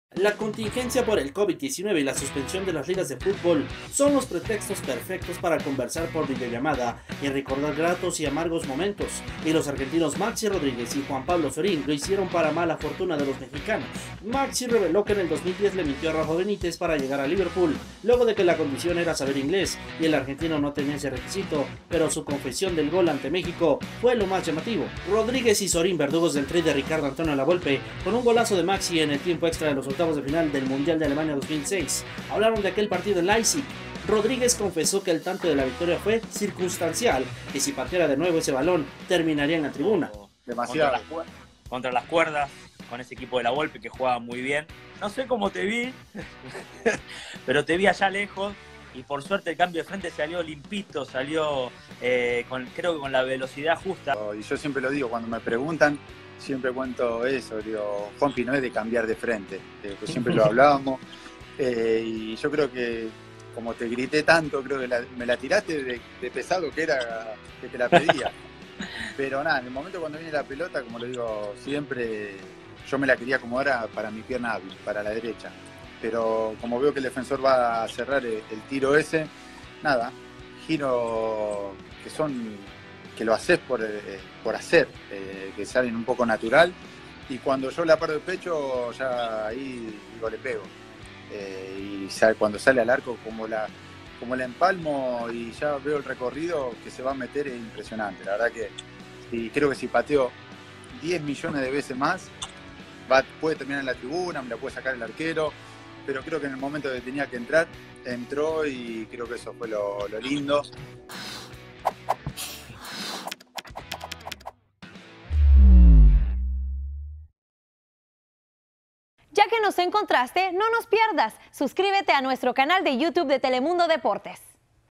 La confesión del verdugo del Tri de La Volpe en el Mundial de Alemania 2006 | Telemundo Deportes
El golazo de Maxi Rodríguez en los octavos de final acabó con el sueño de México de llegar al quinto partido hace 14 años.